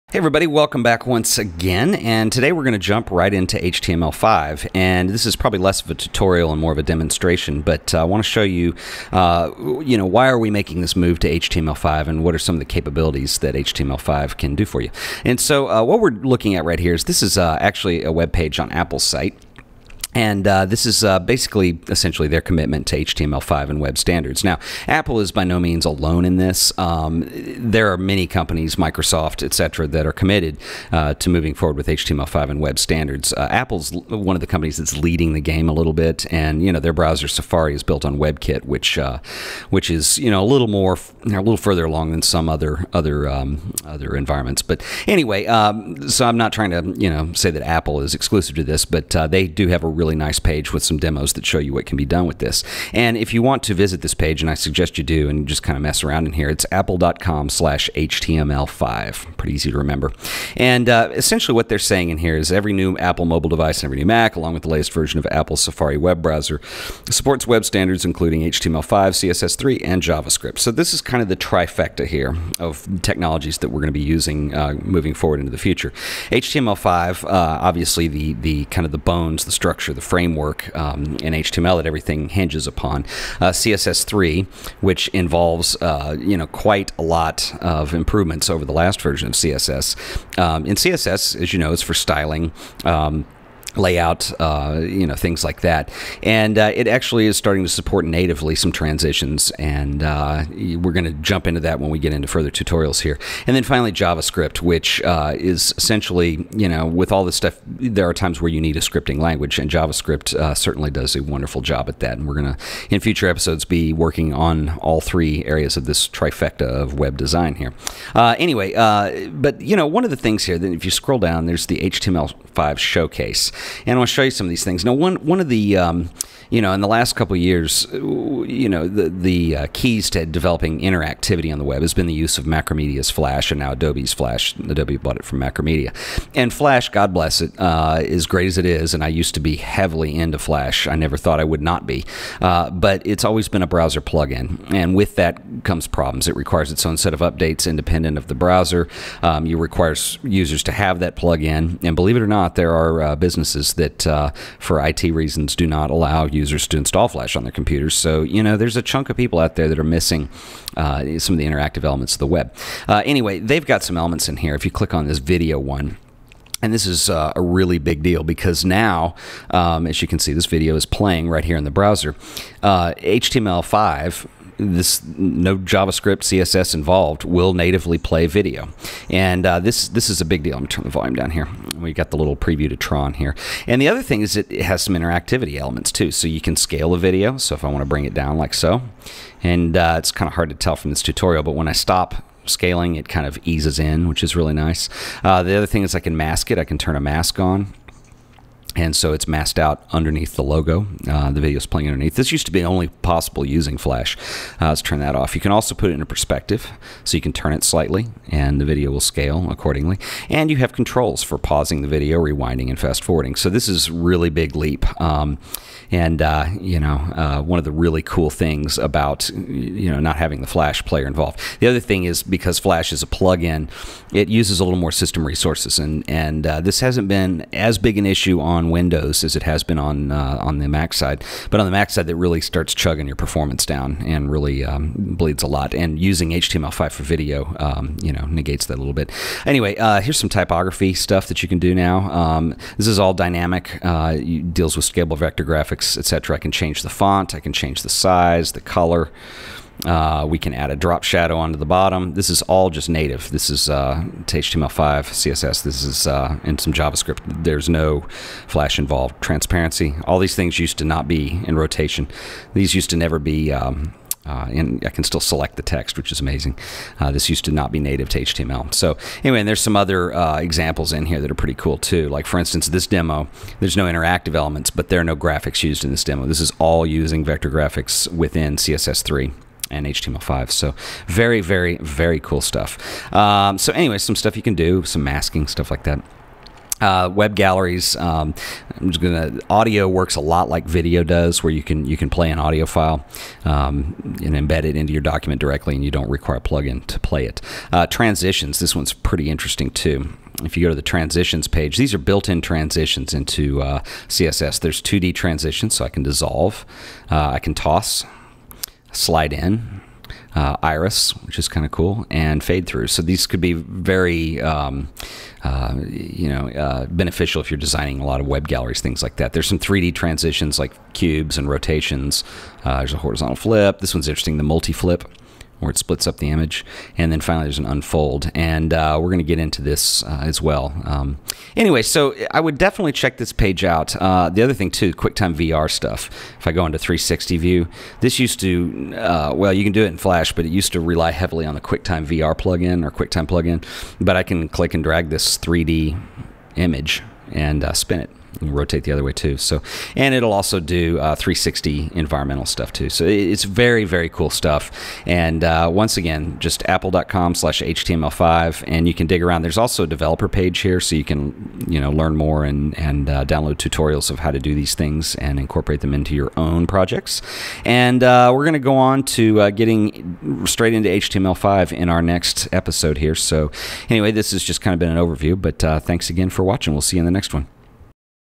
An Overview of HTML5
A quick overview of HTML5 and some of the new possibilities for web development.